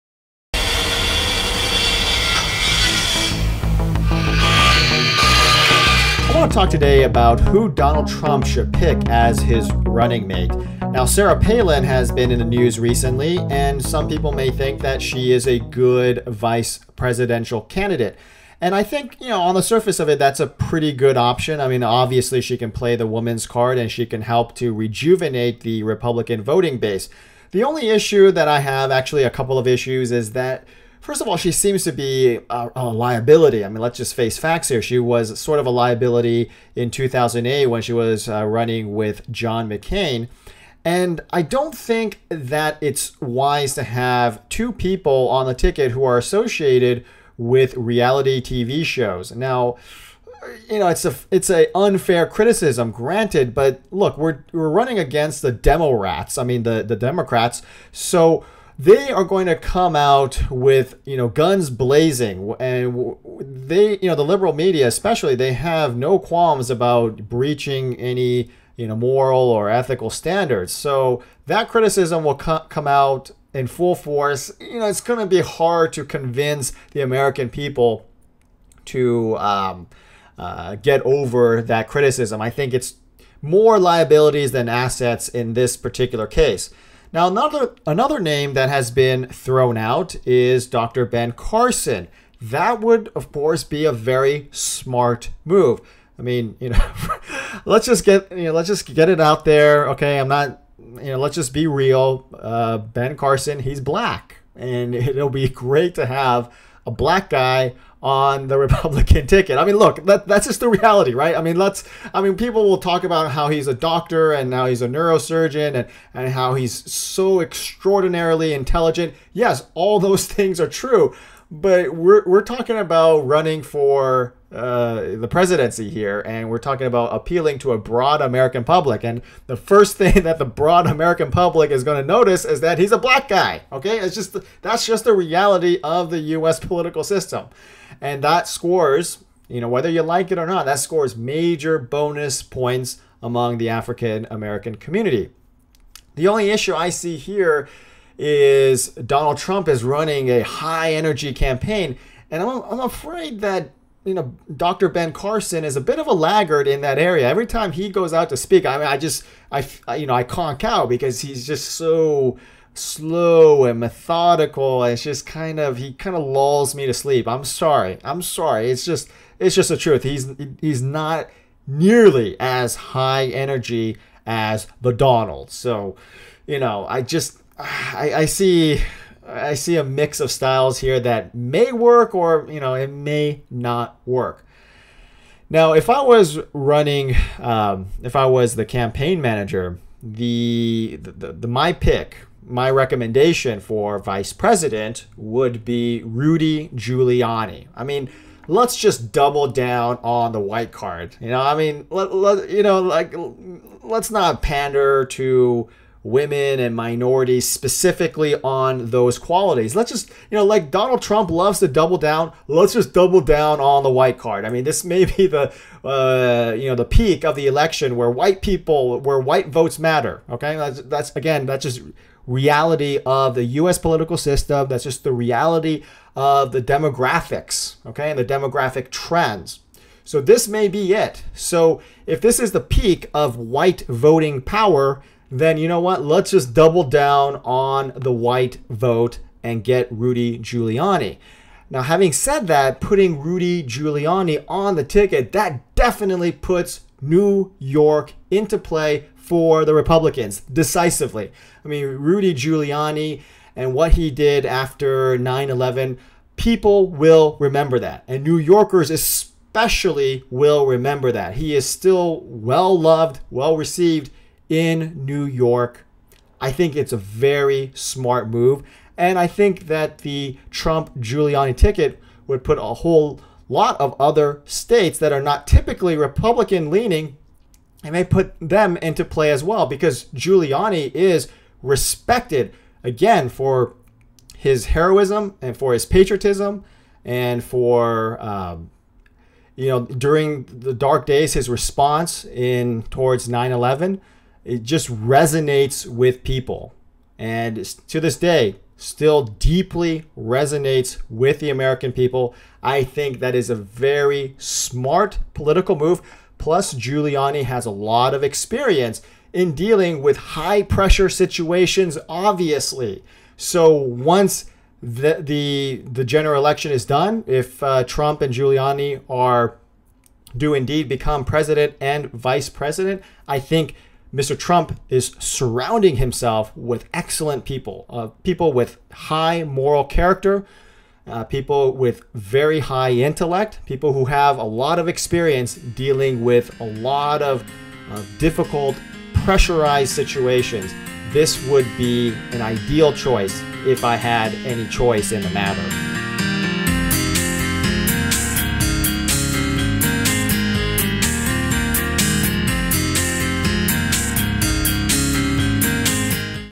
Who Should Donald Trump Pick As Vice President?
Who should Donald Trump pick as his vice Presidential running mate? With Ted Cruz picking Carly Fiorina in a last-ditch Hail Mary, the focus turns to Donald Trump. Fortunately, Mr. Trump has many options. Unfortunately, it's a tough choice, as each potential candidate carries significant pros and cons.

Some of the VP picks could include Sarah Palin, Dr. Ben Carson, and Rudy Giuliani. I go into a quick analysis of my thoughts on each, what they could bring to the table, and who I would pick as the Trump VP if I had any say in the matter.